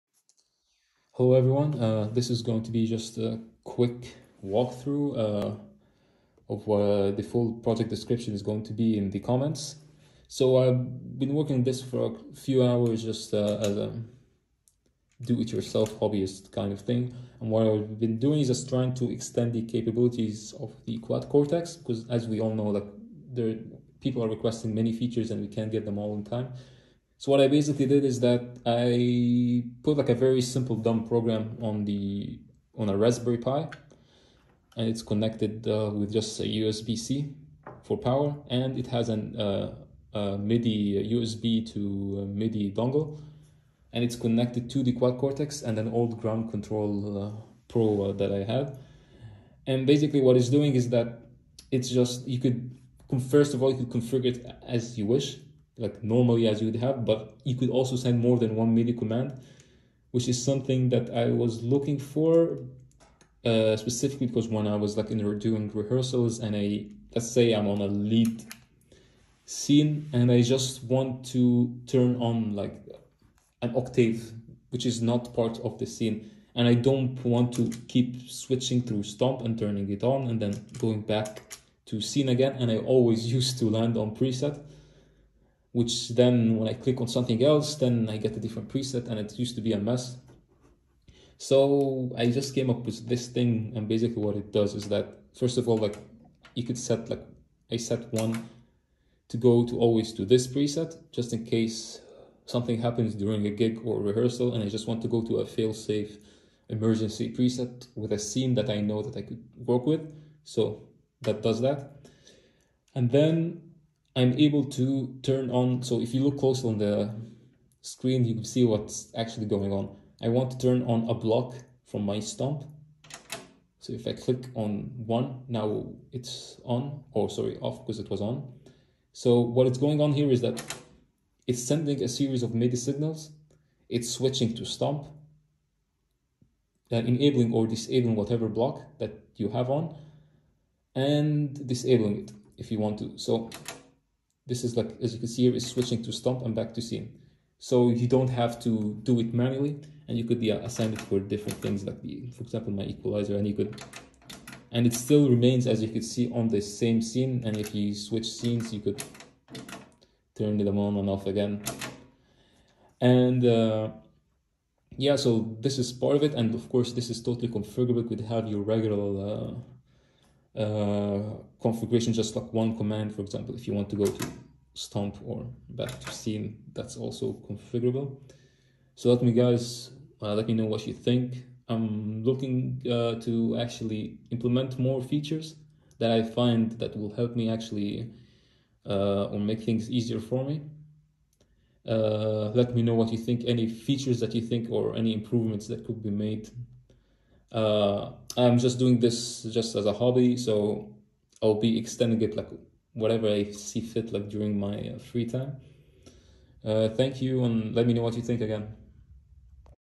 MIDIPilot | Quick intro and Demo
This project was initiated to give extra control capabilities through MIDI to Neural DSP's Quad Cortex. MIDIPilot currently enables you to turn on stomp effects through an external MIDI pedal (Voodoo Lab Ground Control Pro) while "remaining" in scene mode. Right now this program is very simple and what it does is send a series of MIDI CC messages to achieve the result needed. This program of course can be edited to work with any other MIDI pedal.